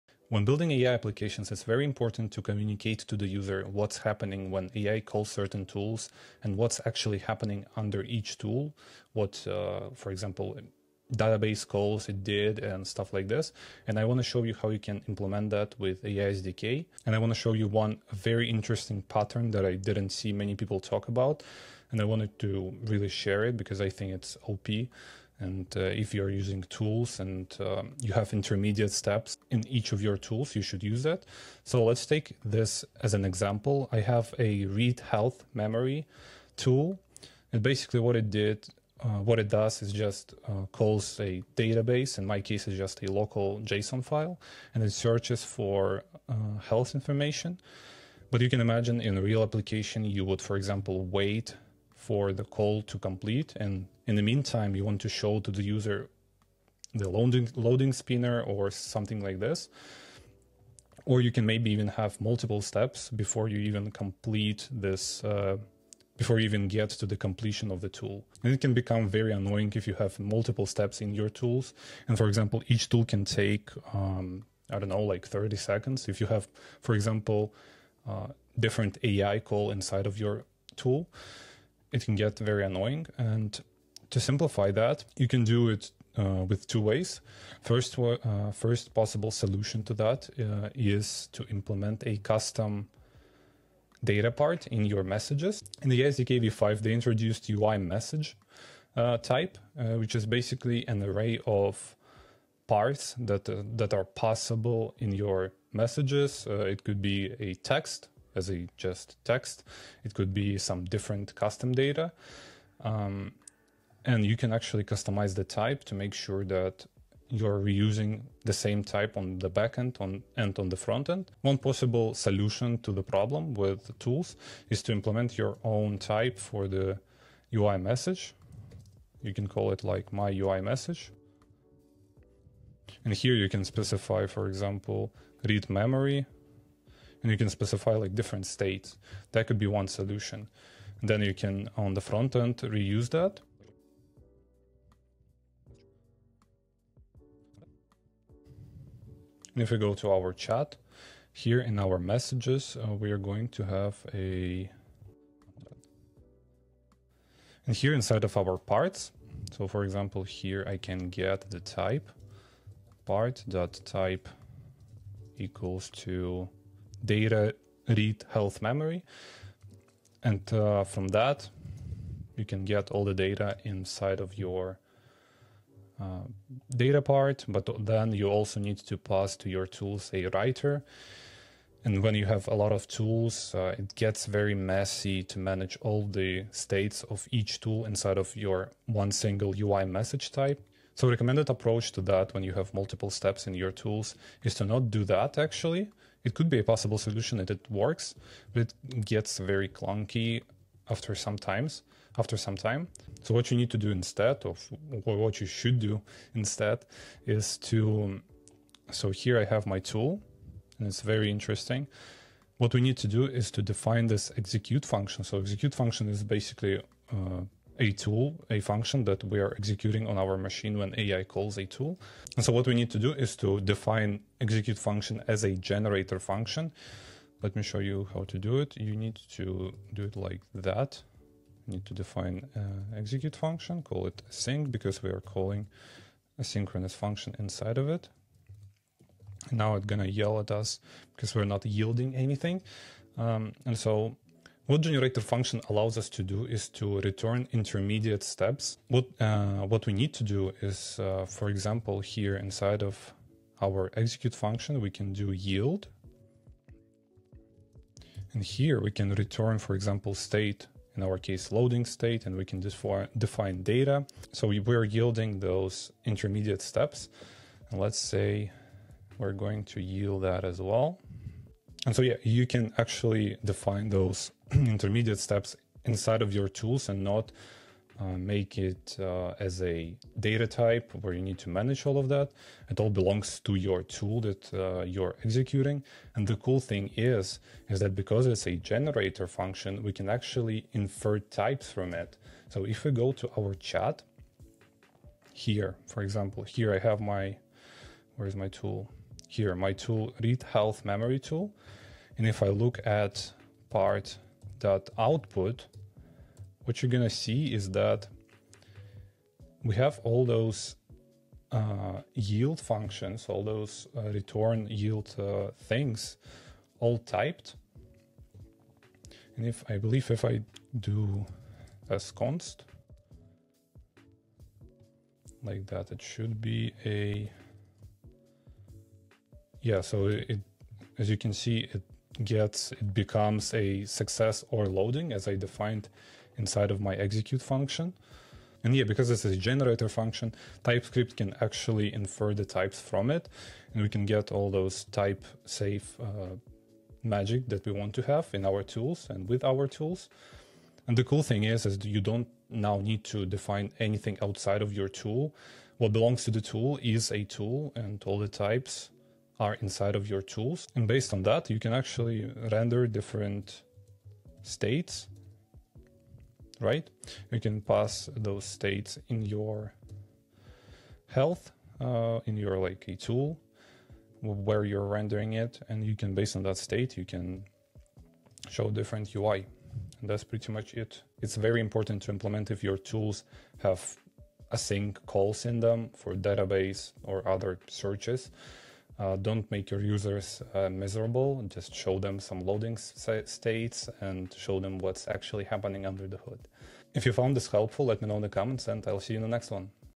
You Didn't Know AI SDK Tools Could Do This
A clever way to display AI tool progress using generator functions